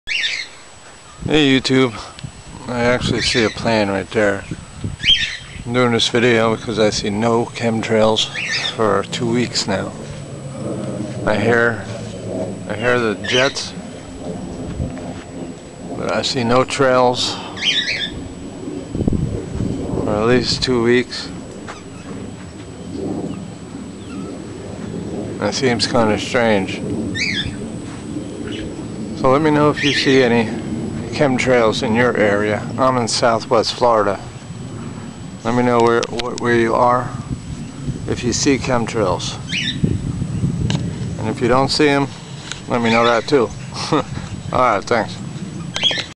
no chemtrails in sight
no chemtrails in sight for 2 weeks,wondering if they are still spraying or not in other states so please do respond thank you
this way we can see who is being sprayed and what to look for in the weather patterns.